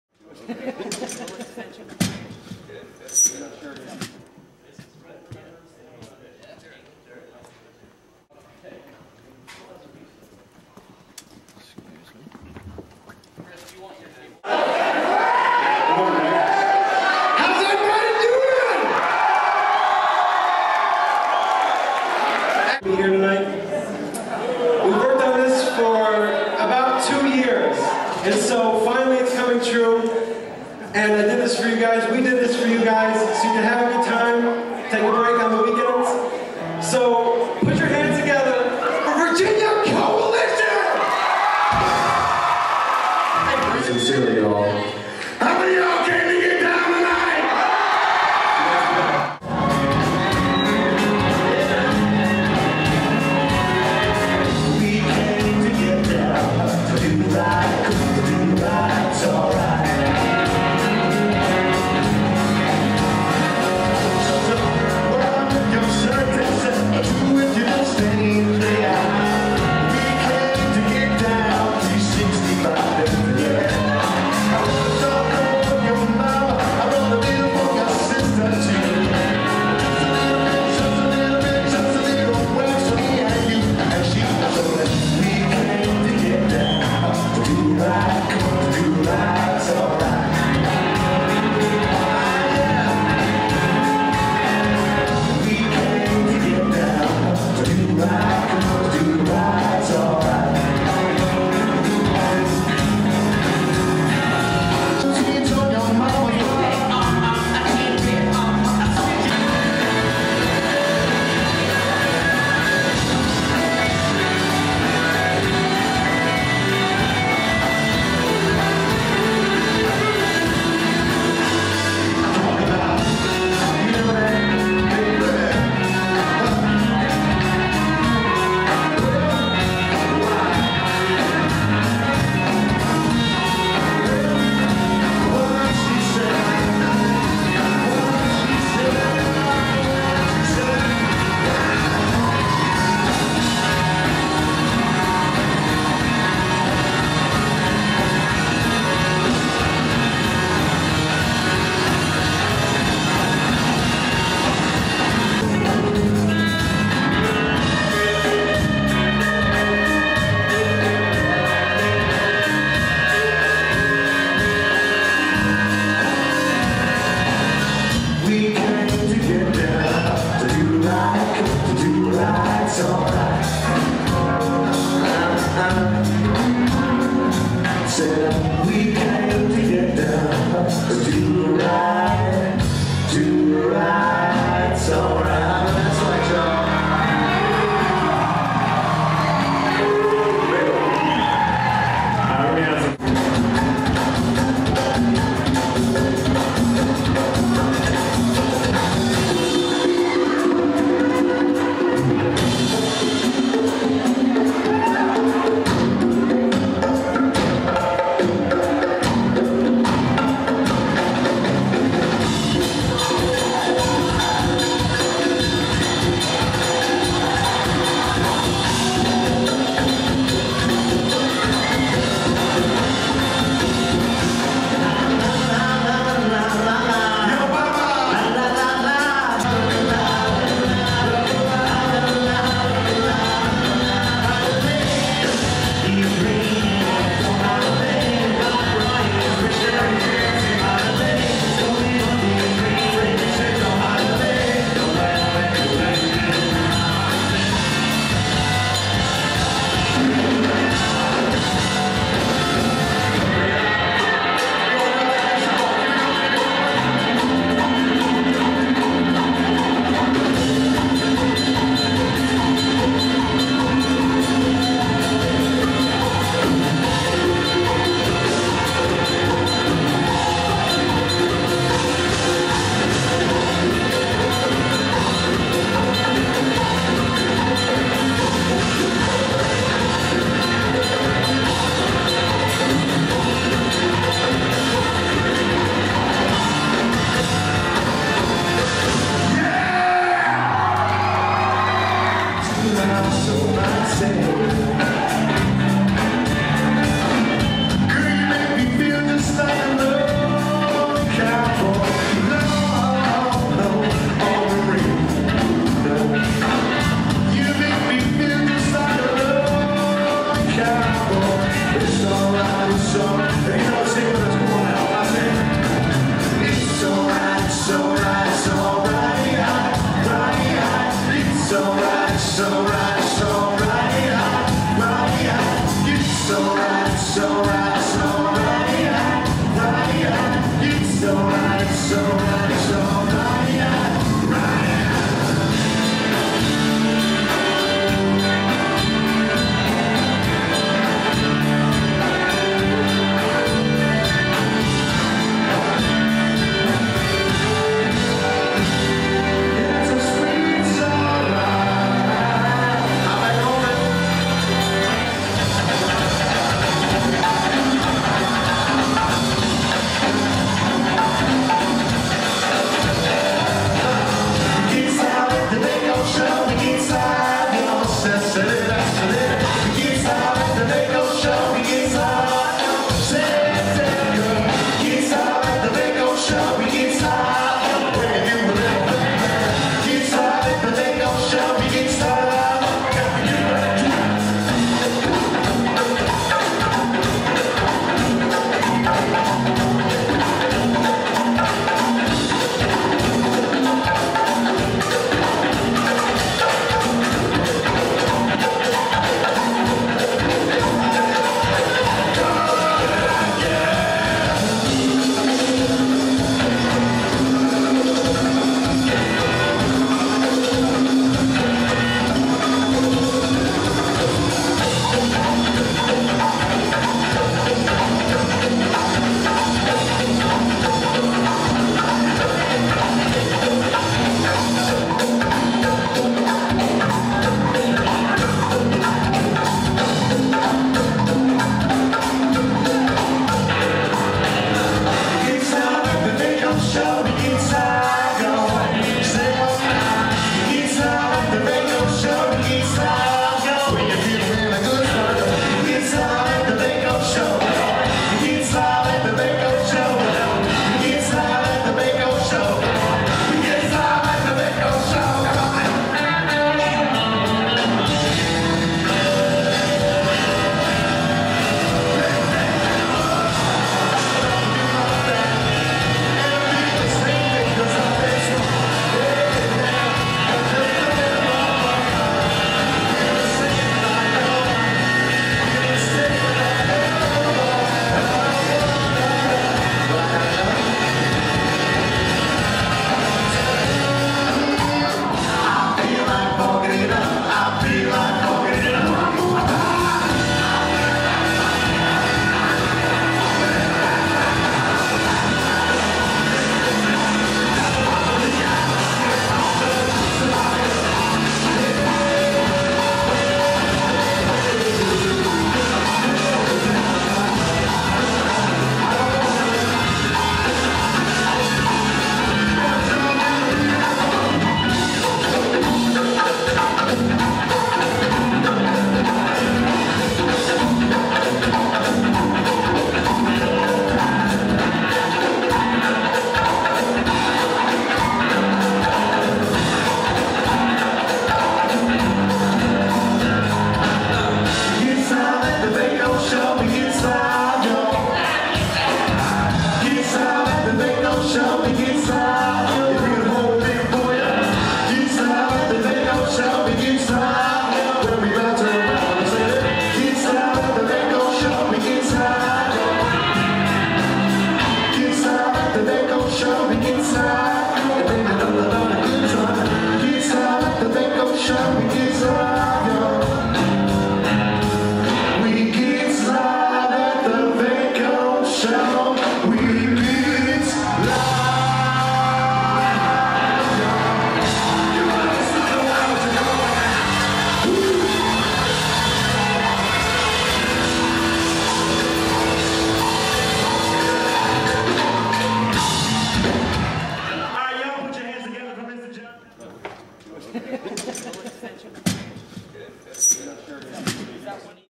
Virginia Coalition (Western Reserve 2003)
Virginia Coalition performed a special concert at Western Reserve Academy, Hudson. Ohio on March 7, 2003.